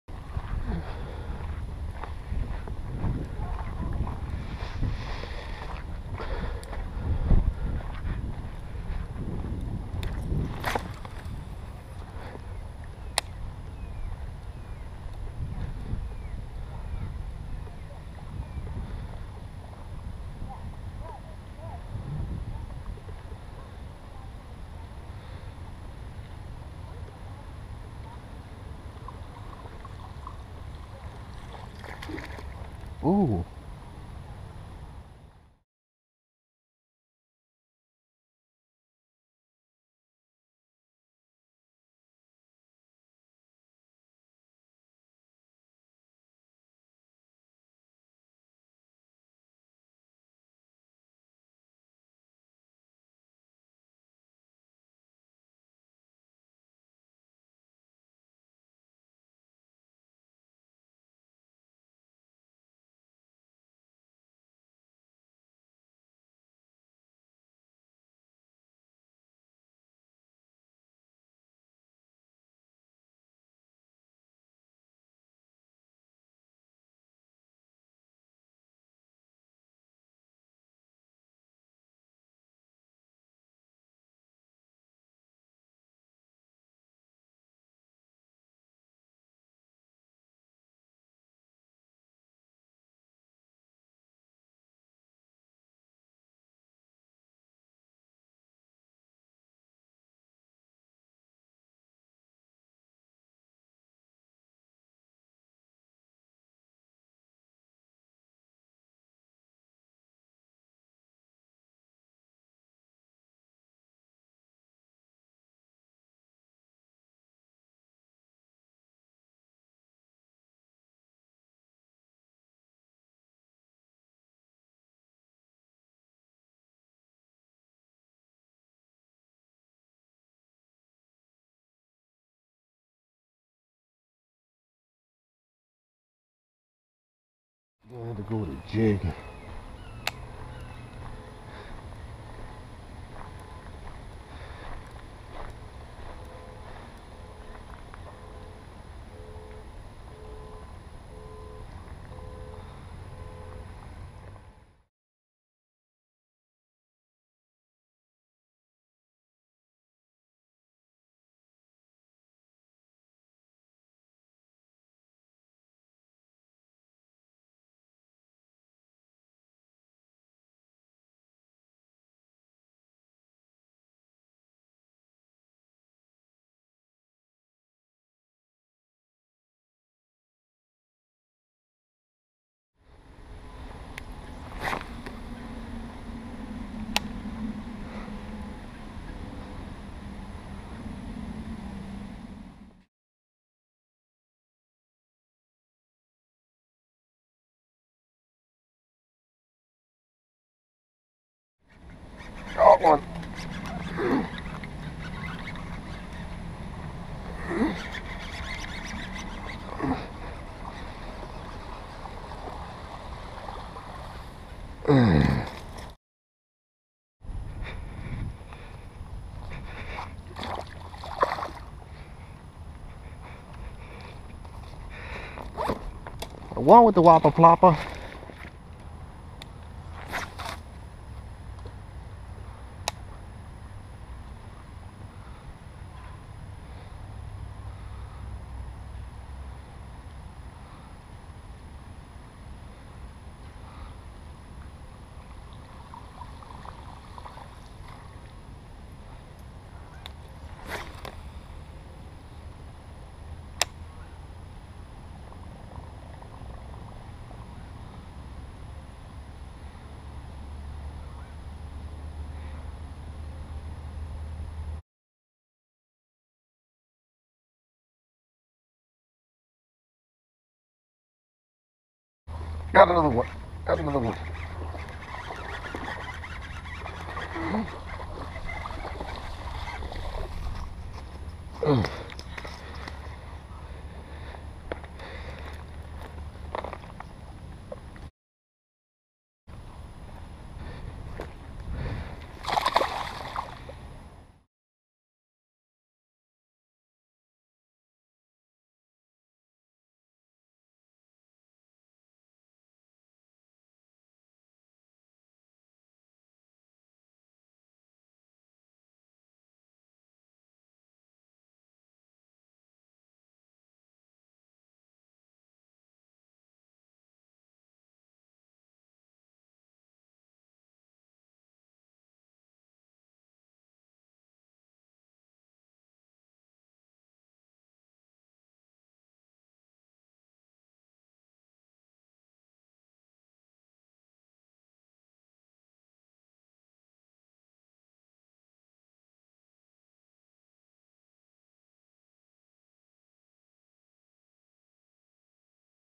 Bass Fishing 9/24/2018
Got my GoPro Hero 3 to turn on again. So I went out Bass fishing and used a Whopper Plopper 110. Missed 3 & Landed 2 nice 2lbs. Skip to 3:00 for when I catch my bass.

Abu Garcia Pro Max Baitcaster
SpiderWire Stealth Superline 20lbs Fishing Line
River2Sea Whopper Plopper 110 Munky Butt